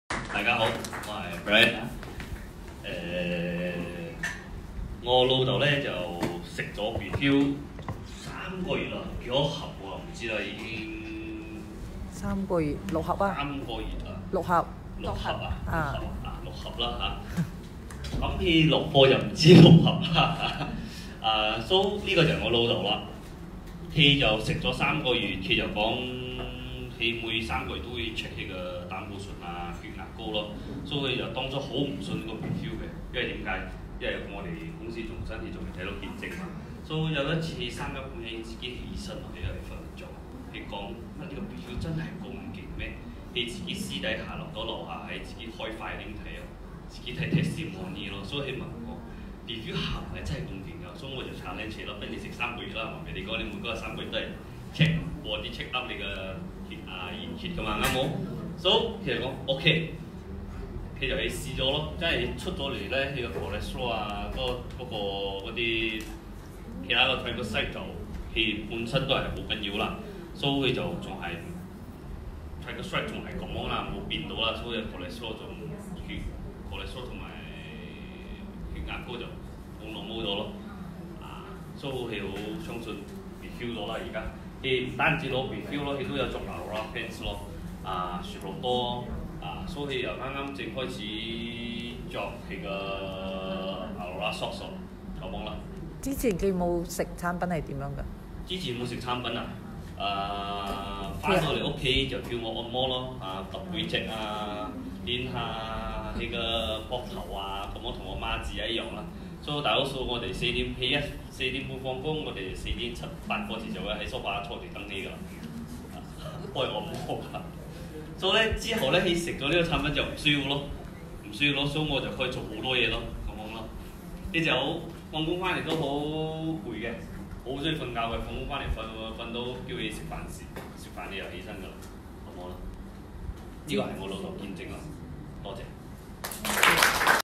Brian Shim sharing his father’s testimony after taking BEFIL for 3 months, cholesterol level impro
Brian Shim sharing his father’s testimony after taking BEFIL for 3 months, cholesterol level improved and bloodpressure also back to normal . He has regained his energy and lessen his shoulder pain.